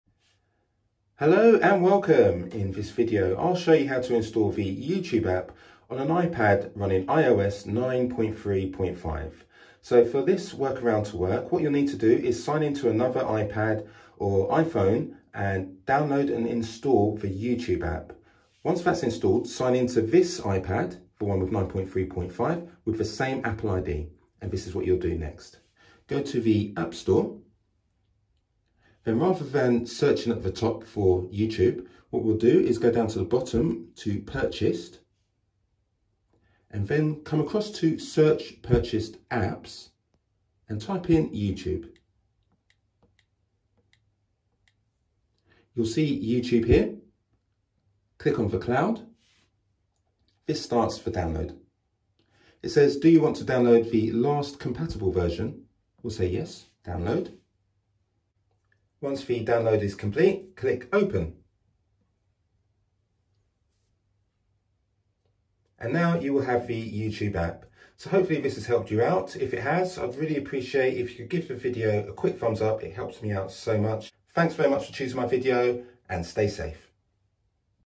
(Not Working Since Update, Sorry) How To Install YouTube on Old iPad 9 3 5
Here's how to install YouTube on an older iPad with IOS 9.3.5. You can still use the YouTube app on your iPad with 9.3.5 and this video will take you step by step on how to do this.

iPad Pro 2020 (Wi-Fi + Cellular)

iPad Air 2020 (Wi-Fi + Cellular)

iPad Stand

iPad Keyboard Case

iPad Slim Bluetooth Keyboard